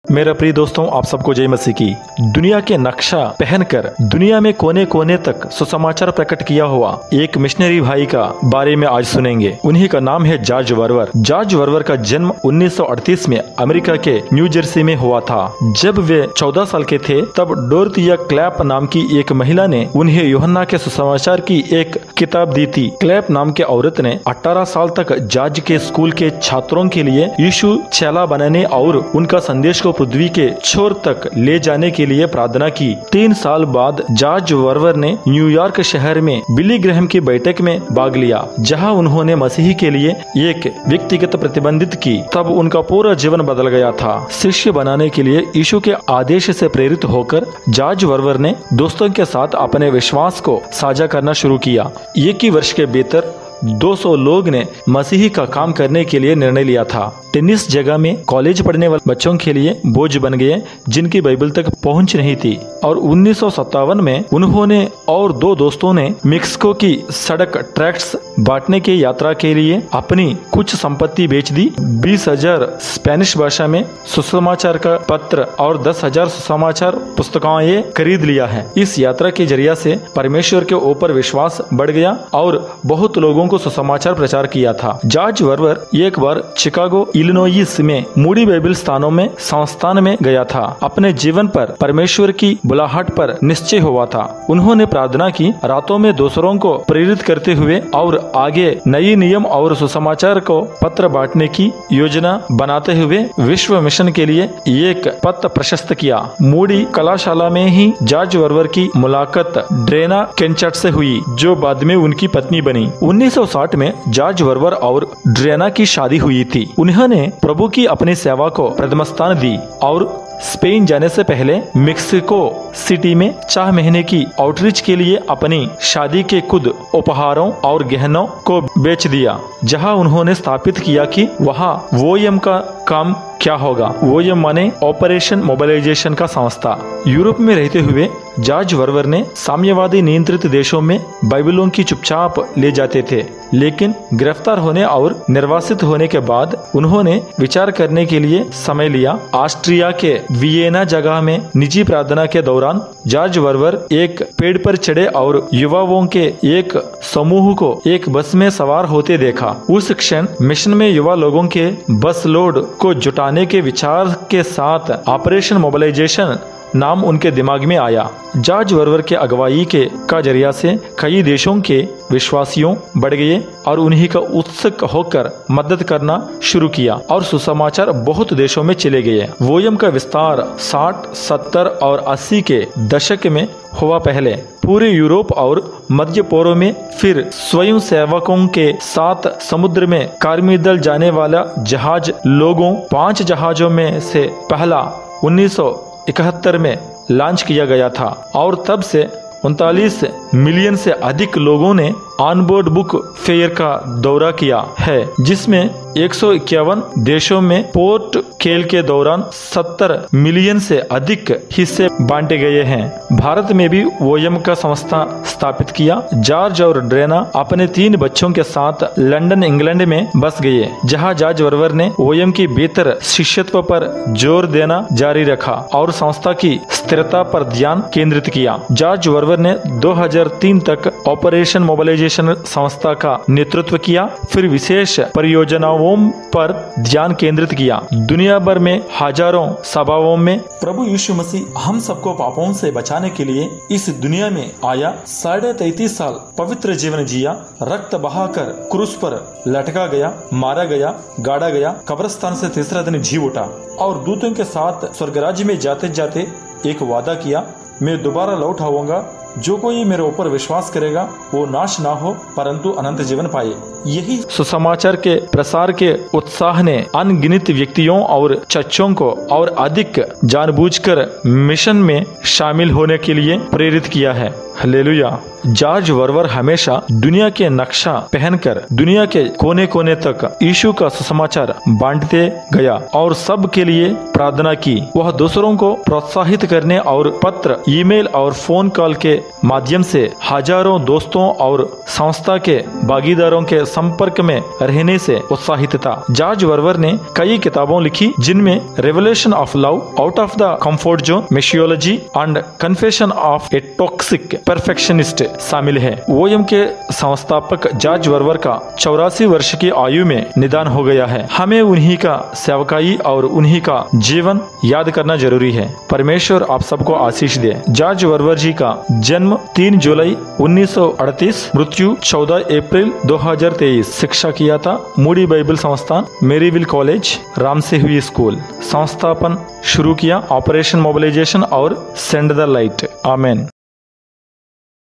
George Verwer # Missionary Biography # Hindi # EP12
George Verwer was an American evangelist and founder of Operation Mobilisation, a Christian missions organization.
Verwer wrote several books on various Christian themes. He was a passionate advocate of radical discipleship
as the only legitimate option for people who believe in Jesus. His recent work was in India. Wikipedia Born: 3 July 1938
Died: 14 April 2023, London, United Kingdom Spouse: Drena Verwer (m. 1960) Organizations founded: Operation Mobilisation, Send the Light
Education: Moody Bible Institute, Maryville College, Ramsey High School